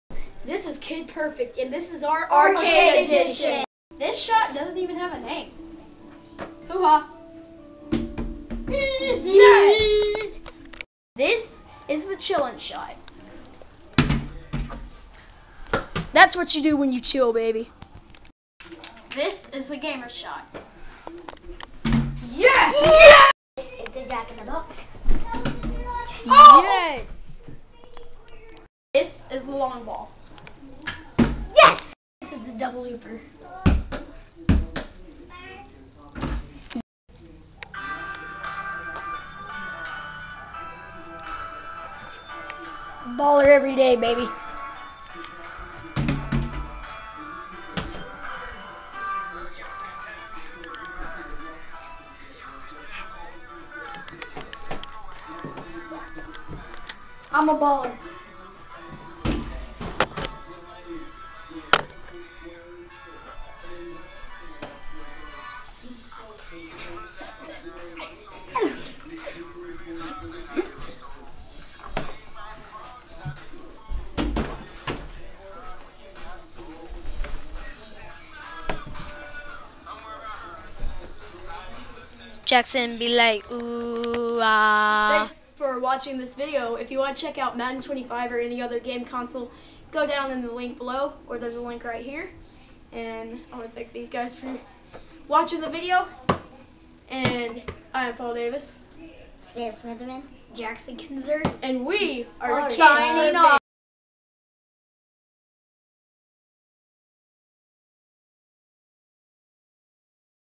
Kid Perfect: Arcade Edition
We hit the neighborhood arcade an try to knock down some shots.